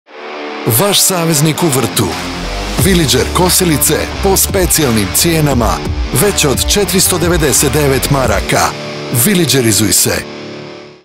VILLAGER® kosilice - TV reklama
Vaš saveznik u vrtu

Villager kosilice po specijalnim cijenama, već od 499 KM

Vilidžerizuj se